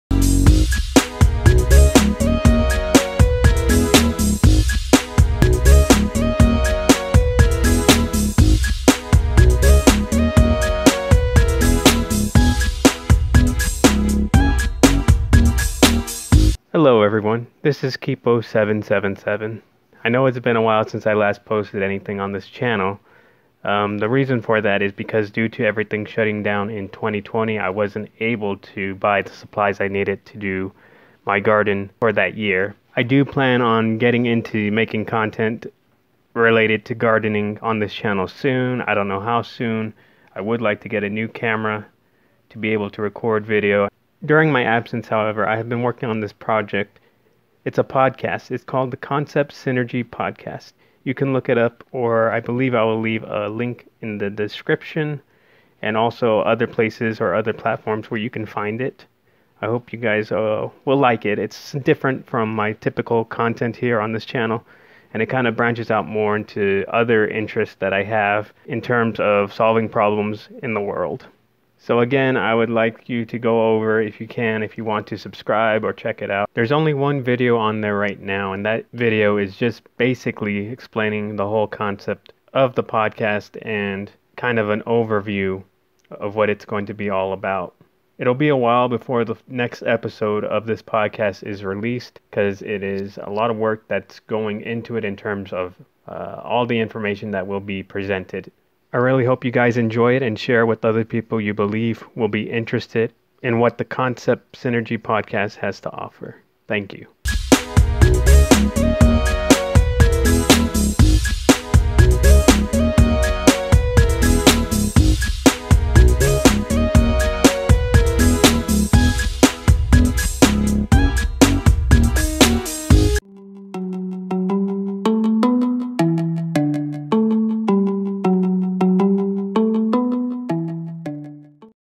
Update and New You Tube Channel
Hello everyone, I have been absent due to everything shutting down in 2020 and not being able to get supplied that I needed for gardening last year. I hope to get back to making garden related content soon. During that time of absence I have been working on a project. Part of what came out of that project is a new You Tube channel and podcast called Concept Synergy Podcast. It's about concepts, idea, topics and more that I believe can help provide solutions for problems I see in the world.

It may be a while for the next episode. There is still a good amount of work to be done.

Thanks for your patience! I hope you all enjoy!